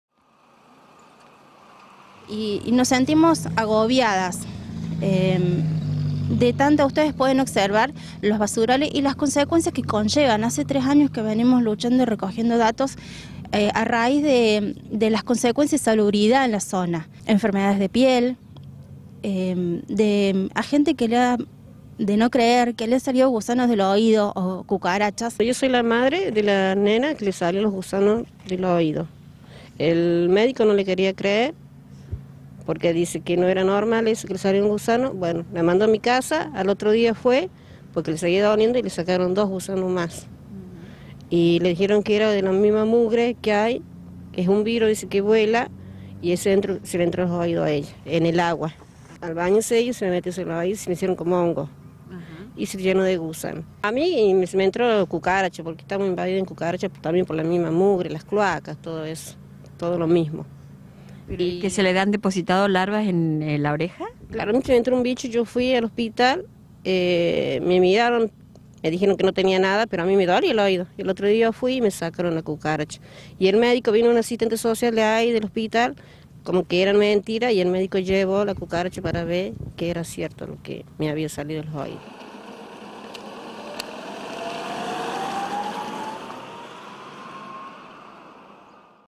En la seccional Quinta el Estado no existe
Los vecinos dicen haber sido olvidados por las autoridades. La Red Social de la Quinta trabaja para mejorar su calidad de vida.